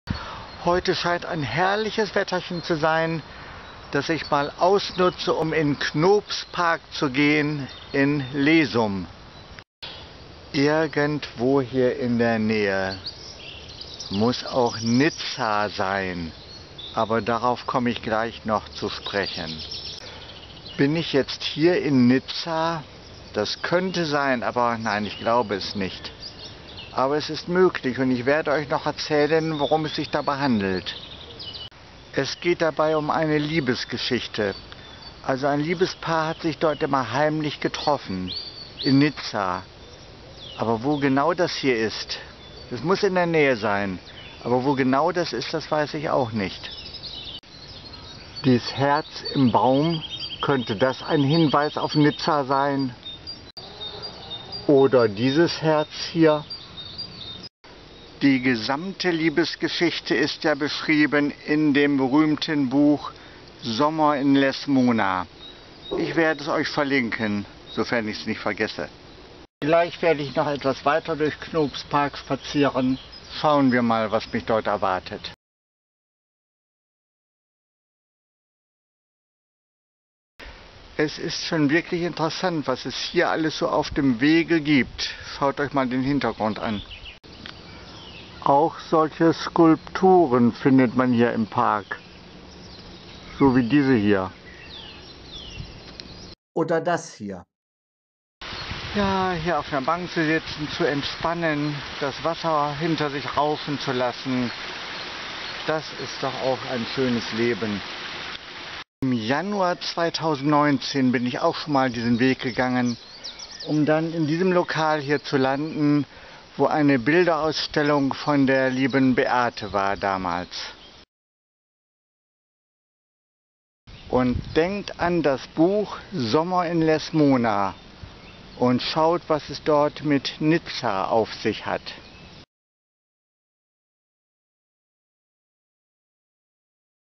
Knoops Park an der Lesum
Nizza - was hat das mit Knoops Park zu tun?
Lies das Buch "Sommer in Lesmona" und du wirst das Geheimnis lüften.
Ansonsten zeige ich in diesem Video einige Eindrücke aus Knoops Park.